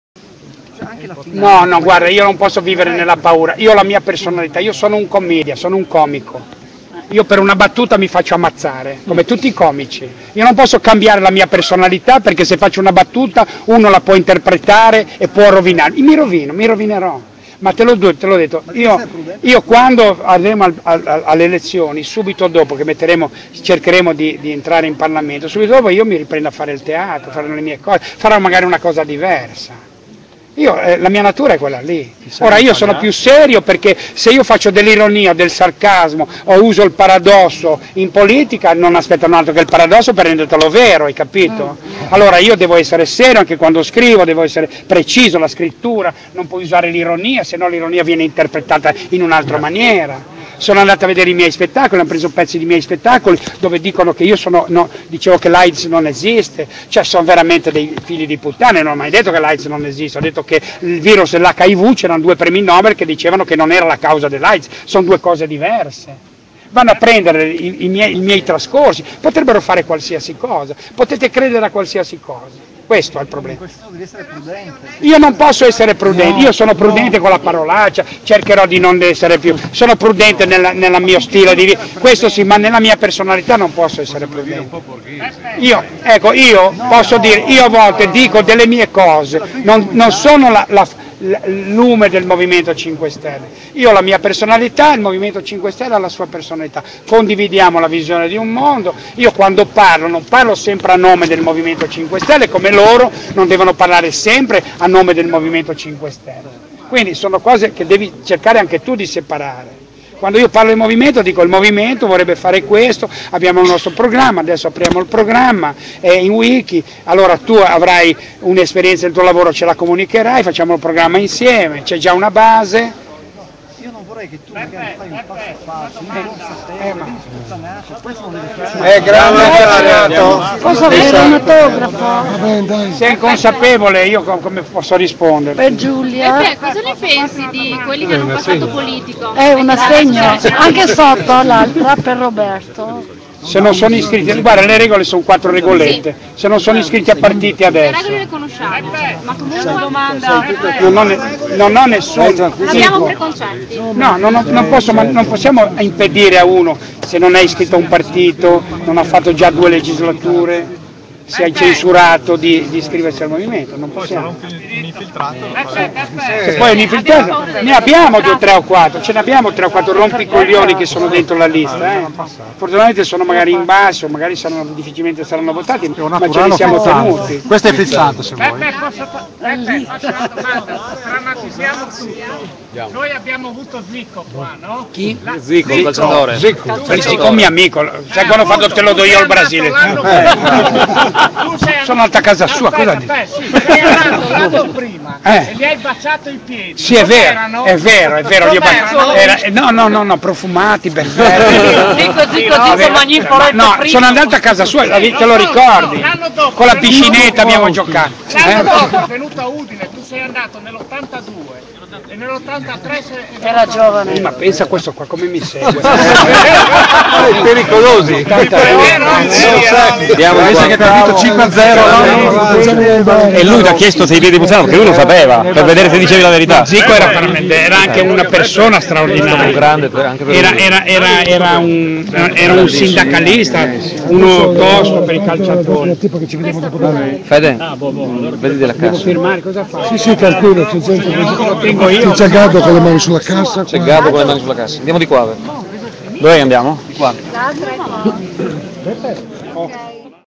Beppe Grillo dietro le quinte. 2/2
Beppe Grillo...molto interessante. Dalle espulsioni ai sondaggi, dal rapporto con Gianroberto Casaleggio alle strategie politiche del Movimento 5 Stelle. Dopo aver spiegato che i voti delle primarie in Lombardia sono stati "pochi" dal momento che il movimento non ha potuto fare pubblicità, il comico genovese rivendica il fatto che le parlamentarie possono essere considerate il "primo esperimento al mondo".

Autofinanziati
Un esperimento tutto fatto "molto velocemente e senza soldi". Per quanto riguarda Casaleggio, invece, Grillo non vuole che i militanti lo considerino un guru: "È solo una persona onesta e per bene che ha deciso di impegnarsi per cambiare le cose". Chiacchierando con i militanti di Udine, poi, Grillo parla anche delle espulsioni di Giovanni Favia, di Federica Salsi e degli altri dissidenti.

Il futuro
Quando un militante gli chiede se non sarebbe consigliabile maggior prudenza, vista la strumentalità di alcune critiche, Grillo rifiuta di ammorbidire il suo impegno e con sincerità dice: "No, guarda, io sono un comico, morirei per una battuta, subito dopo le elezioni, che cercheremo di entrare in Parlamento, io riprendo a fare teatro, a fare le mie cose".
Beppe Grillo ad Udine. Firma day 16/12/2012